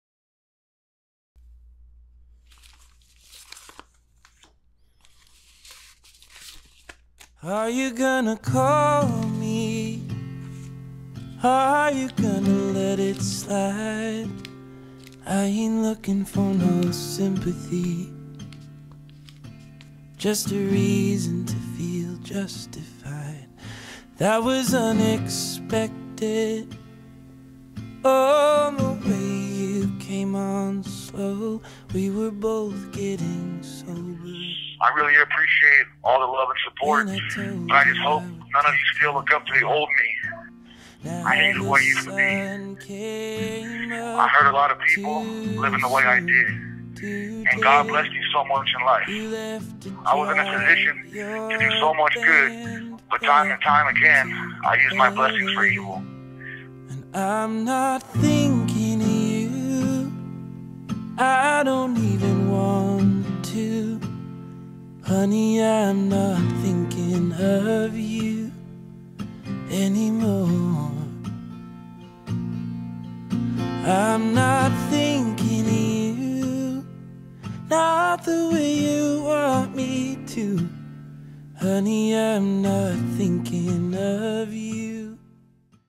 PREVIEW Jon "War Machine" Koppenhaver Exclusive Interview | OUT 9.12.18 | Christy Mack
On July 25th 2018 I got a letter in the mail and it was from Jon Koppenhaver aka. War Machine. After getting to know him more and spending a lot time talking to him I started to see what life in prison has done to Jon. His regrets, his apologies, his new found faith. In an upcoming Interview/Documentary we will look deeper into seeing if prison changed Jon Koppenhaver and his feelings toward Christy Mack.